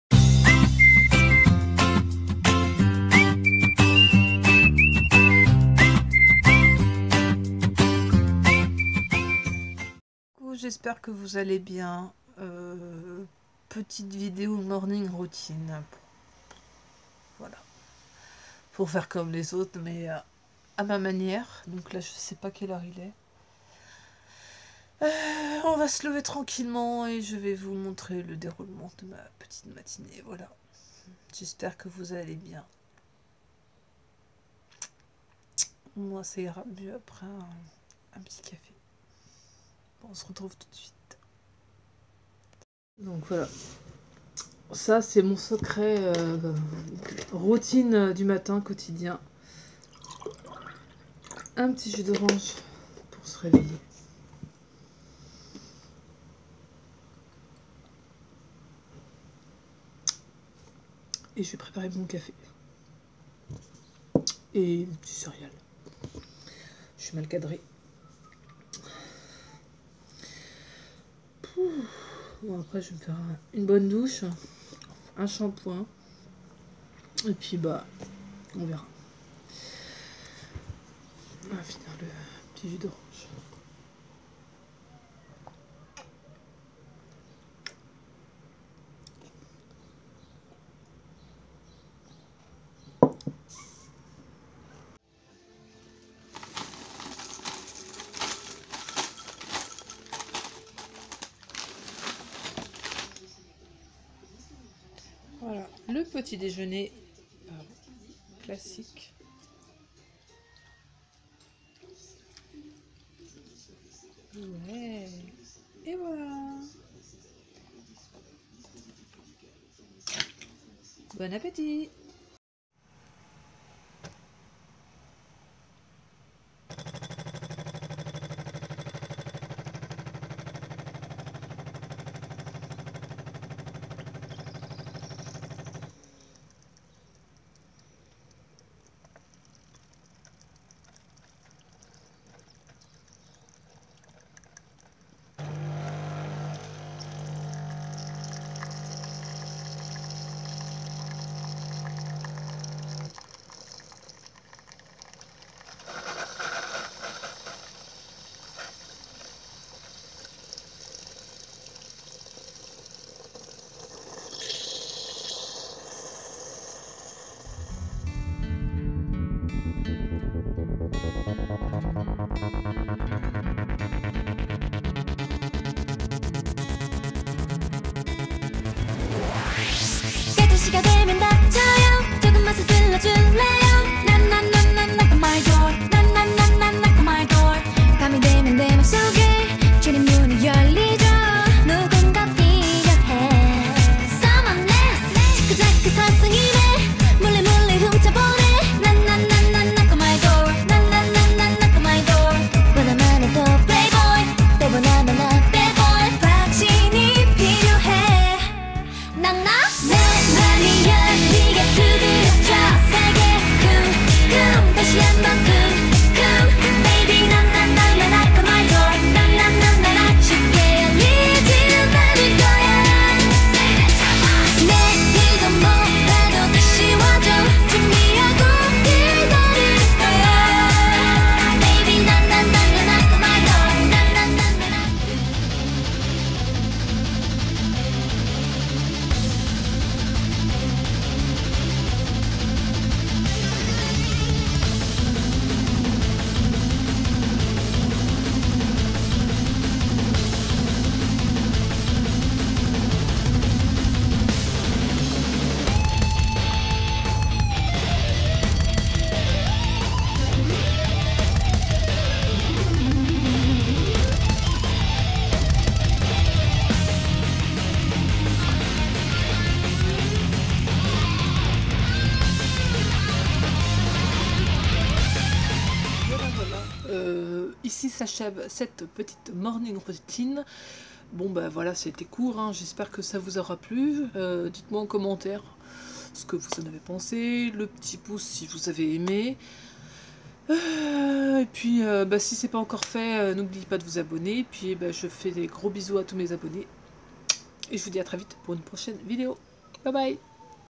Ma morning routine
hello !
Je me lance a faire une morning routine à ma façon. J’espère que ça vous plaira, sans chi-chi et avec un peu d'humour me voici au réveil.